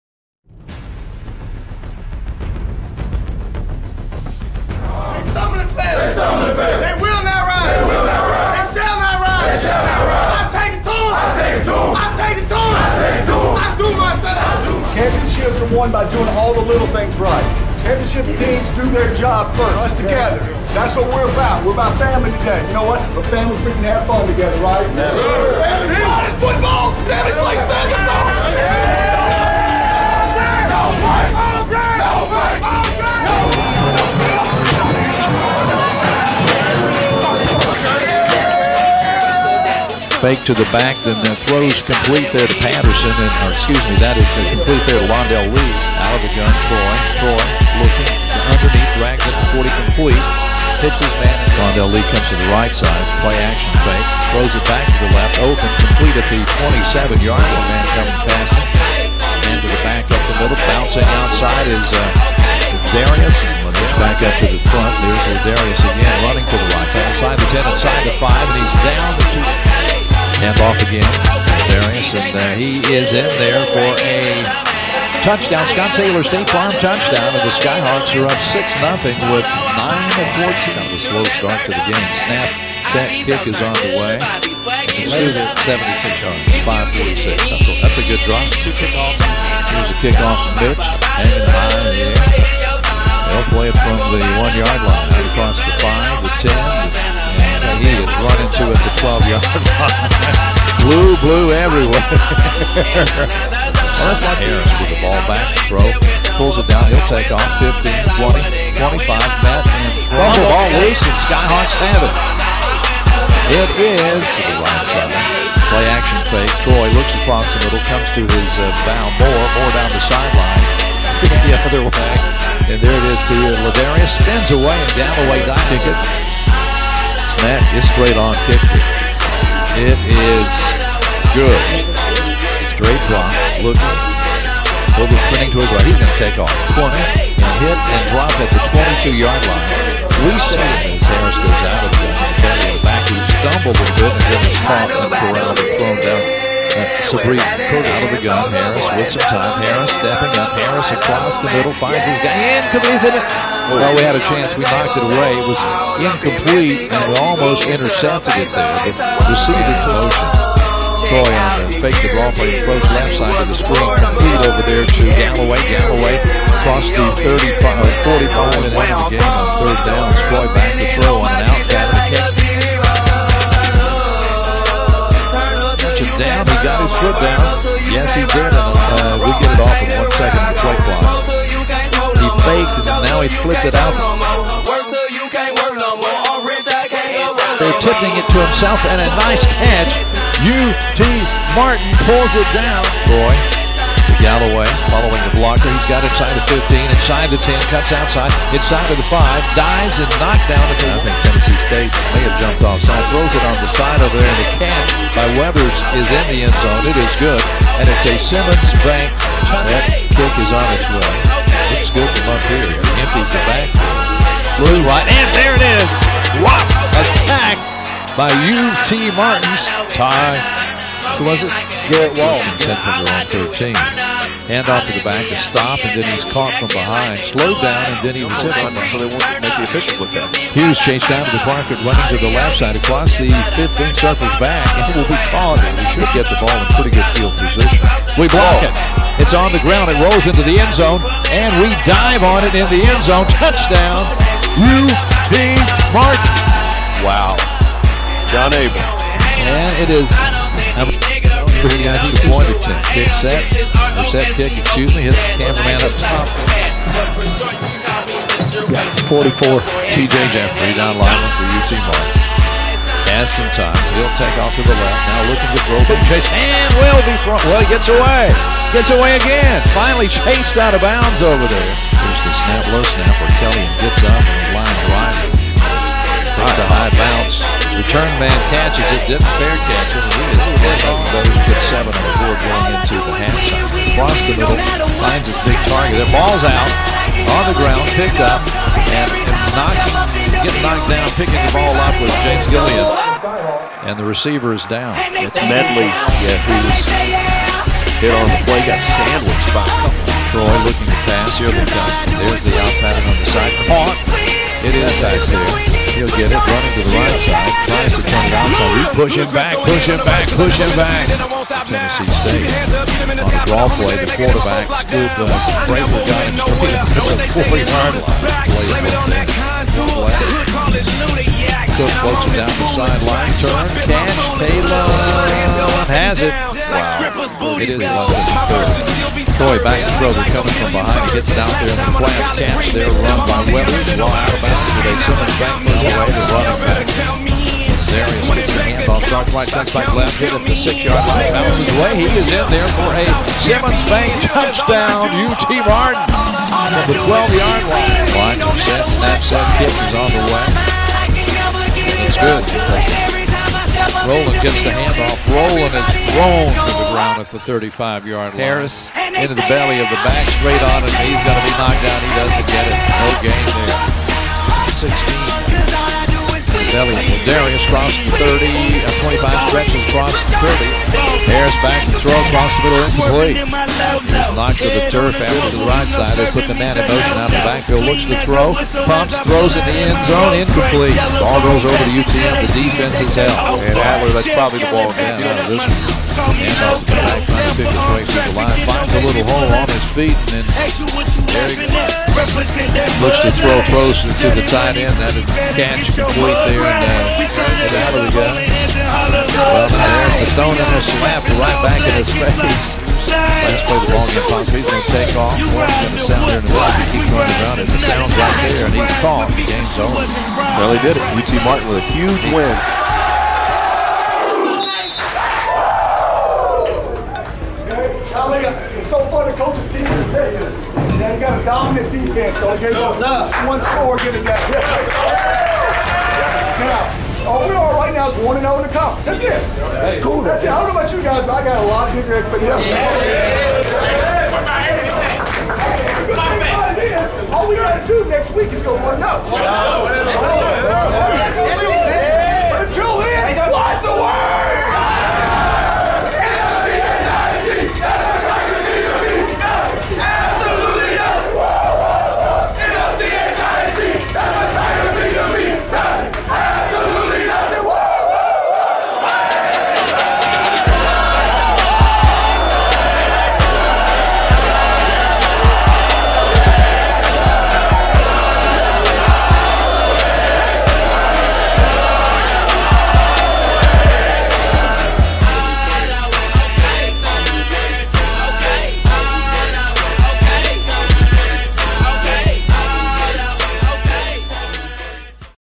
2017 Game 4 - UT Martin vs. Tennessee State
The Skyhawks defeat TN State 31-16 on September 23, 2017.